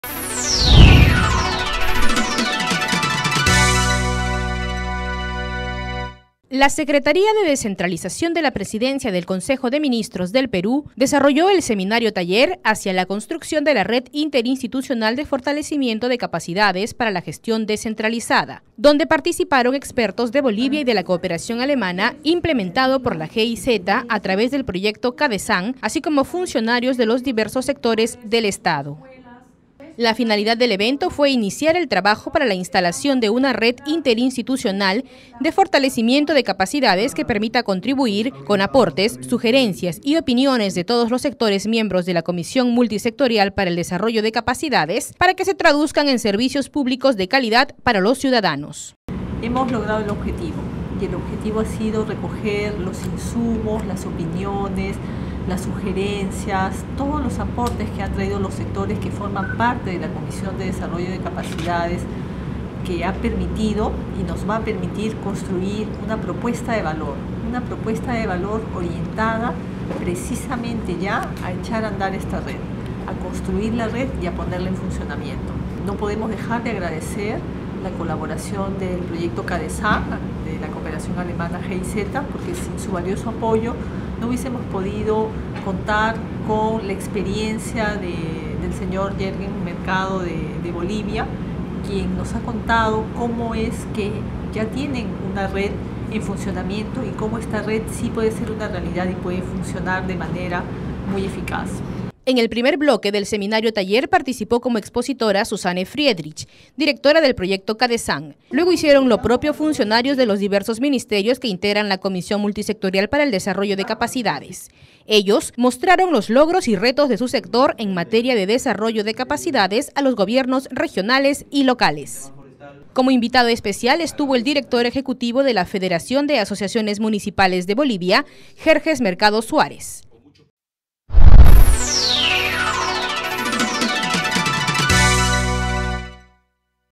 Conforman Red Interinstitucional para fortalecer capacidades de los gobiernos regionales y locales
La Secretaría de Descentralización de la Presidencia del Consejo de Ministros desarrolló con éxito el Seminario-Taller “Hacia la Construcción de la Red Interinstitucional de Fortalecimiento de Capacidades para la Gestión Descentralizada”, donde participaron expertos del país hermano de Bolivia y de la Cooperación Alemana implemenado por la GIZ a través del proyecto CADESAN, así como funcionarios de los diversos Sectores del Estado.